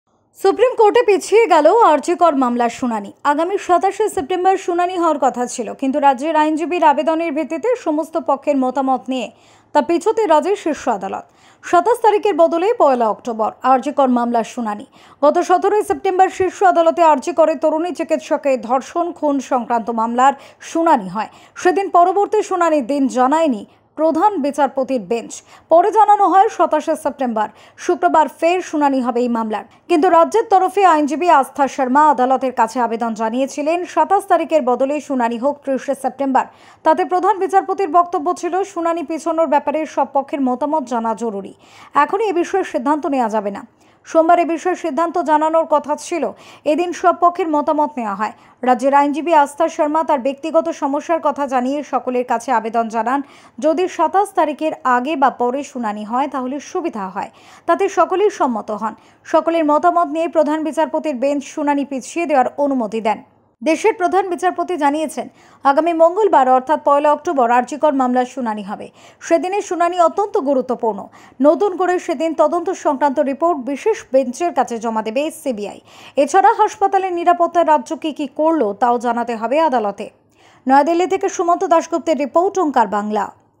শীর্ষ আদালতে পিছিয়ে গেল আরজি কর মামলার শুনানি
সুপ্রিম কোর্টে পিছিয়ে গেল আরজি কর মামলার শুনানি। আগামী ২৭ সেপ্টেম্বর, শুক্রবার রাজ্যের দায়ের করা মামলার শুনানি থাকলেও তা পিছিয়ে গেল। আগামী ১ অক্টোবর, মঙ্গলবার শীর্ষ আদালতে মামলার শুনানি হবে বলে খবর।